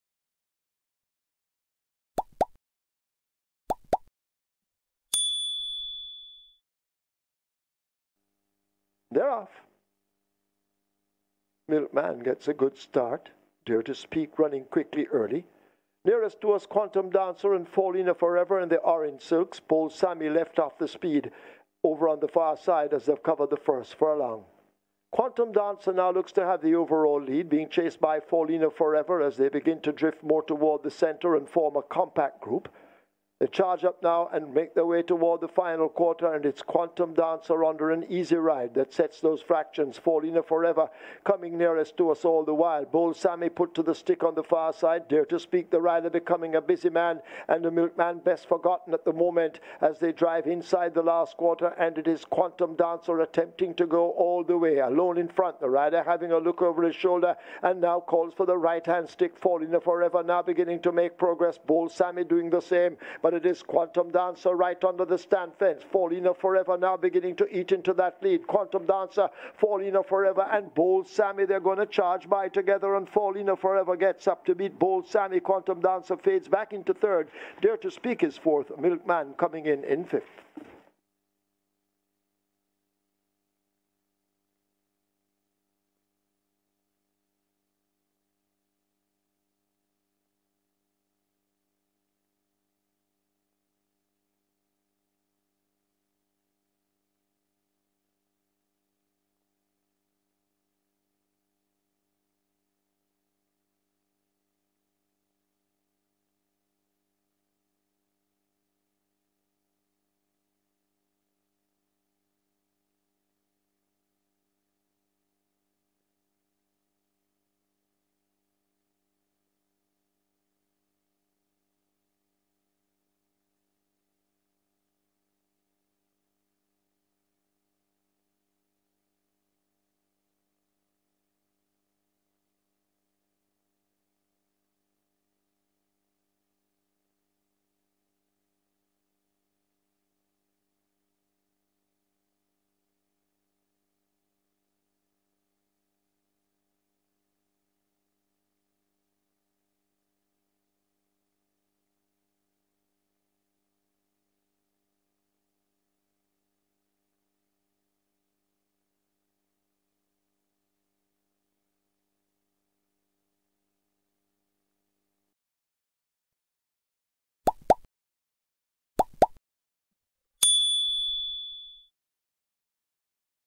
JAMAICA RACING: Faulyna Forever Gives Trainer Lennis Shim First Win, Sat, Jul 8, 2023, 3rd
KINGSTON, Jamaica – One of the new batch of trainers, Lennis Shim saddled his first winner, Faulyna Forever in the Caymanas, Saturday, July 8, 2023, third.
Racing over five furlongs straight in a $400, 000 - $300, 000 claiming call , jockey Anthony Thomas took Faulyna Forever from off the pace inside the final furlong to get home by 1 ¼ lengths.